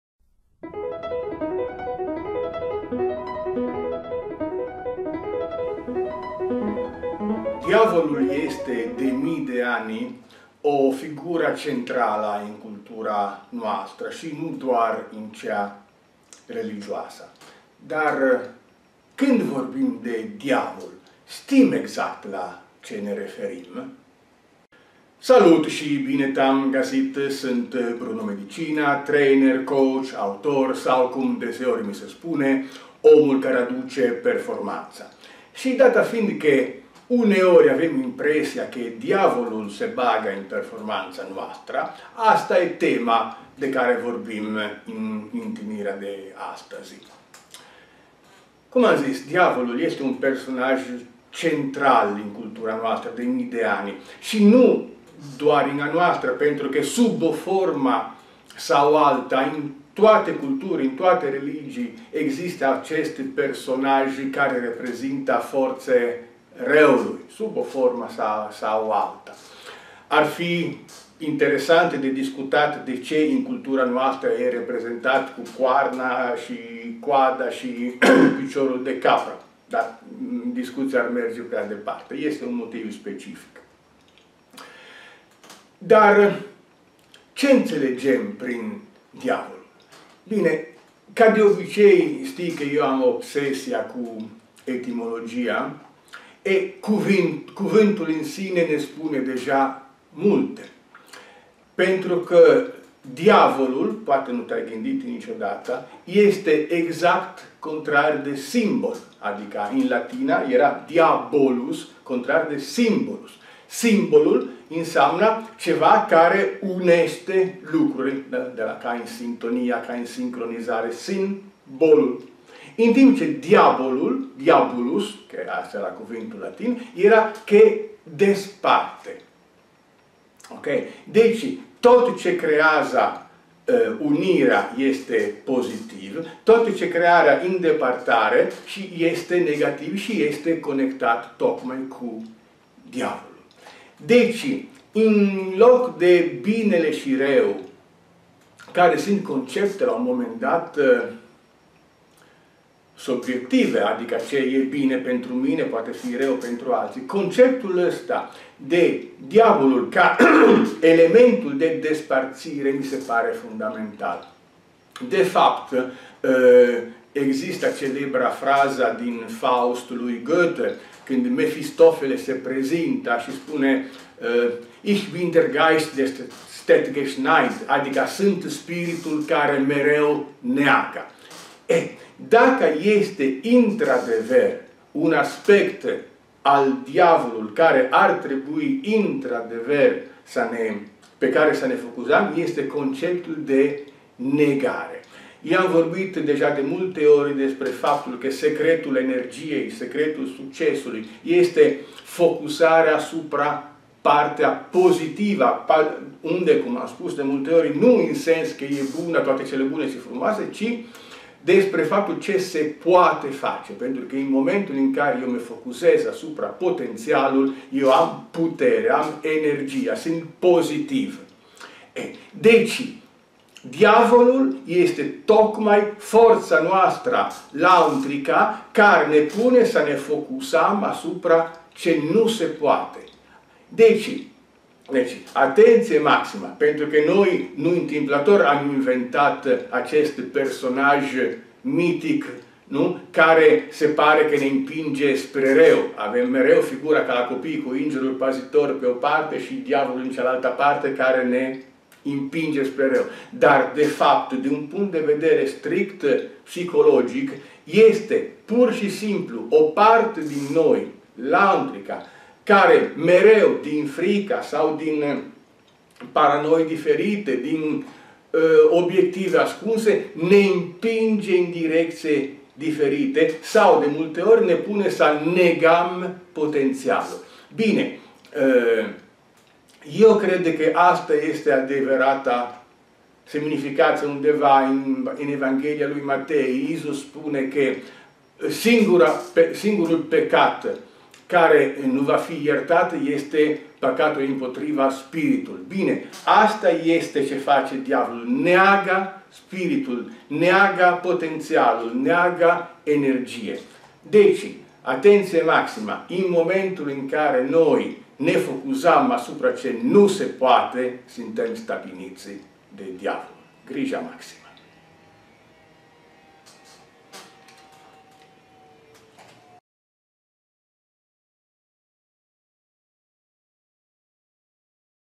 Cine este (sau ce este) Diavolul
Indiferent de credințele noastre personale, de mii de ani Diavolul este o figură centrală în cultura noastră. Dar știm cu adevărat cine sau ce este Diavolul?